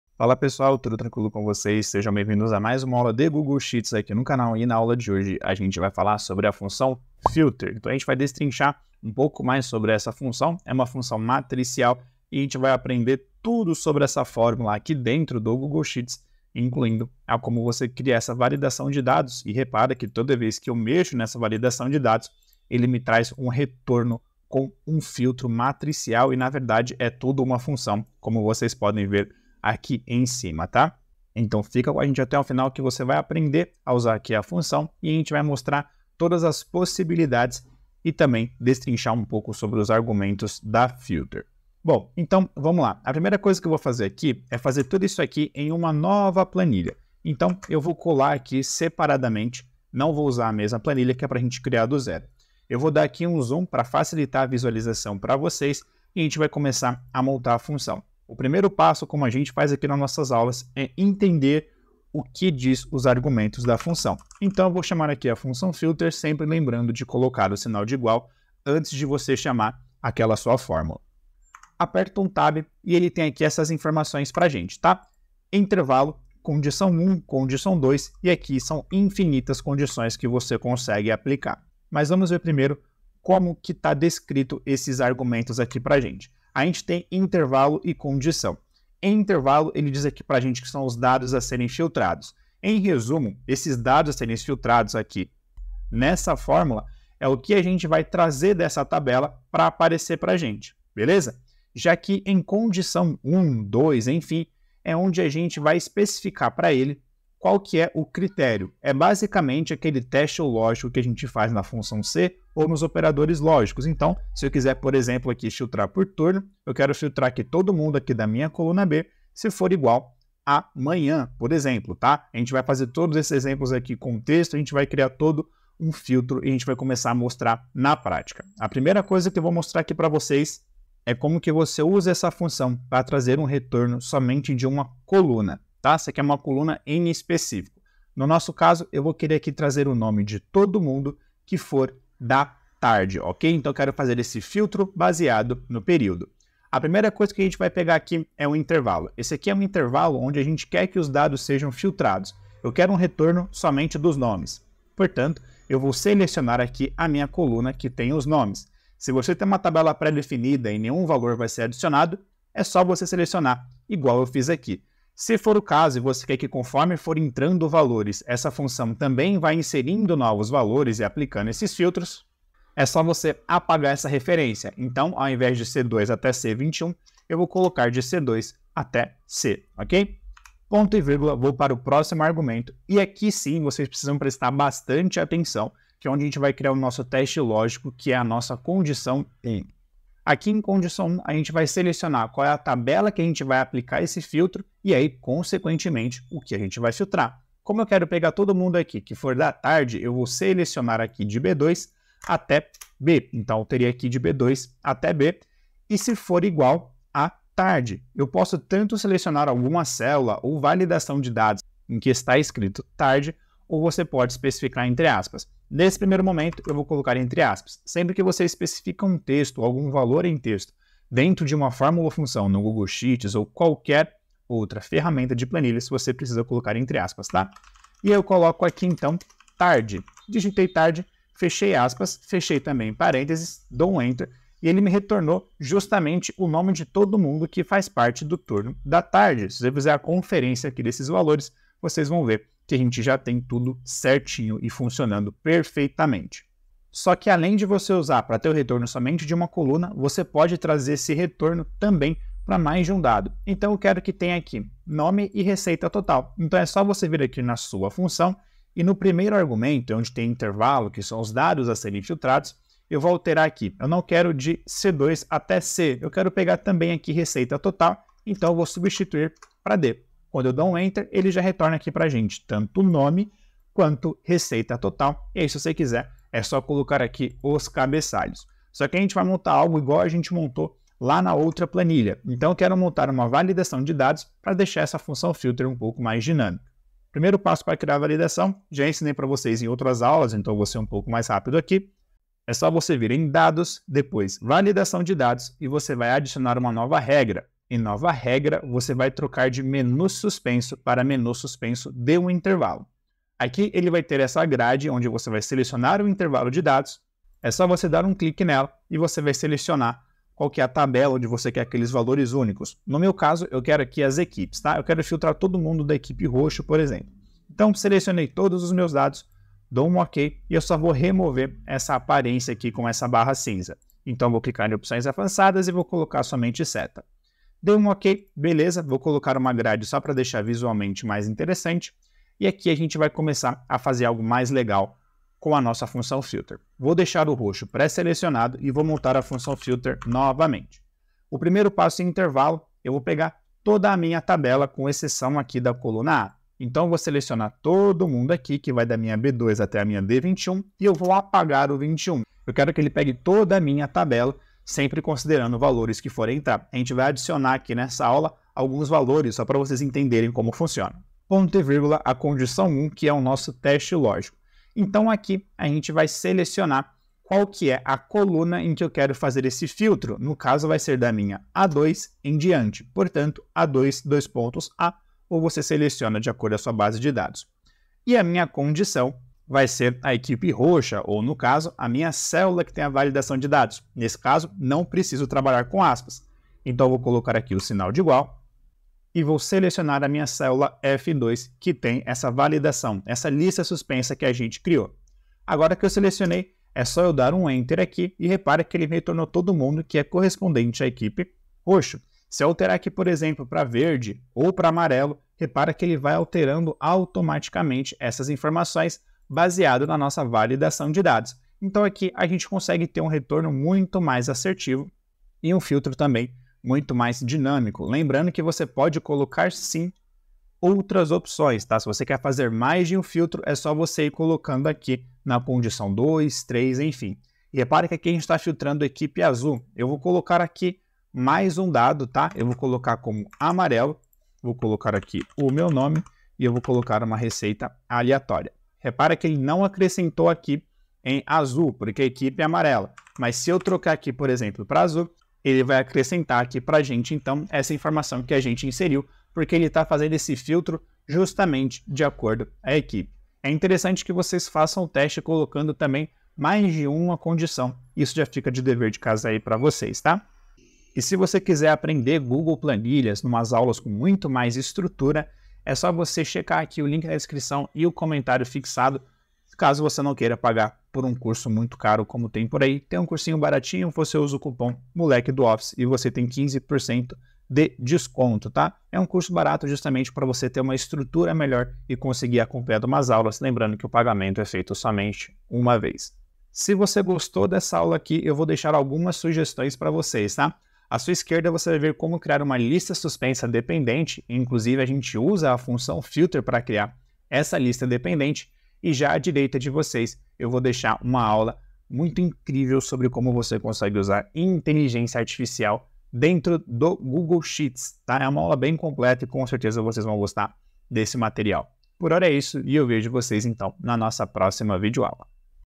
Função FILTER no Google Planilhas - Passo a Passo
A função FILTER no Google Planilhas permite uma análise mais dinâmica e interessante em seus relatório, além de algumas utilidades bem importantes em suas planilhas. Você verá tudo nessa aula completa

📲Nos acompanhe!